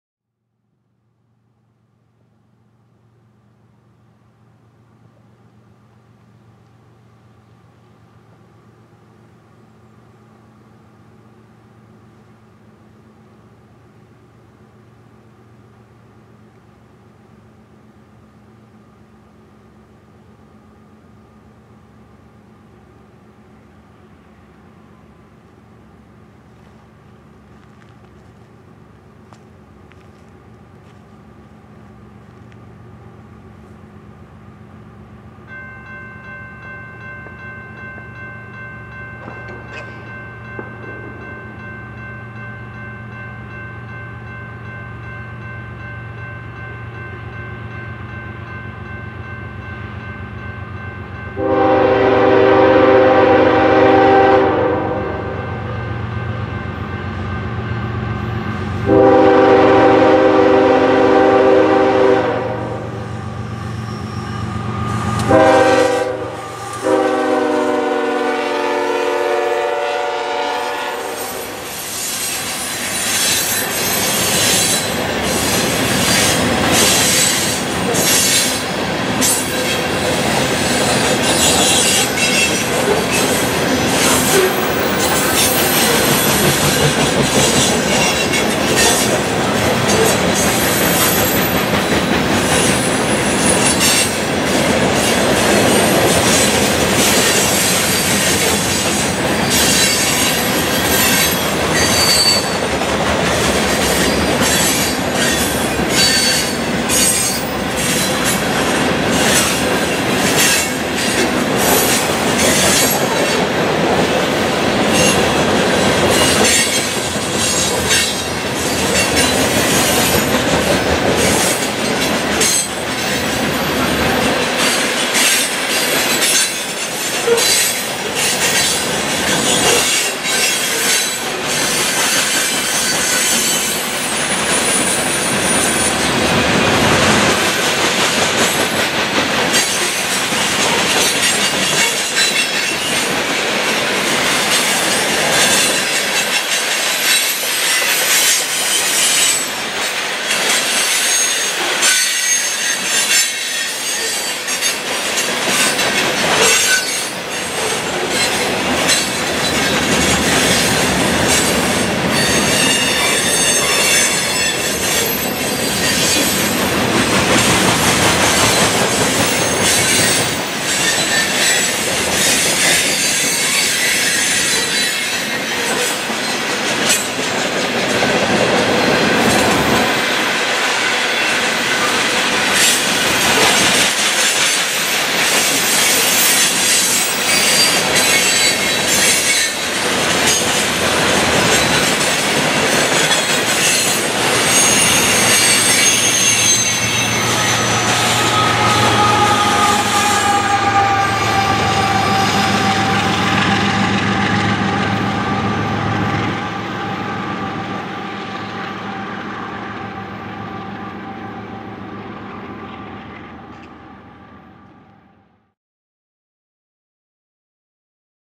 UP Grain train on the BNSF Seattle Sub
UP grain train Northbound through Ridgefield, WA
Date: 1-3-14

Location Ridgefield, WA

Power: UP 7436 AC45CCTE, UP 6480 AC44CW, UP 3824 SD70M, & DPU UP 7659 AC45CCTE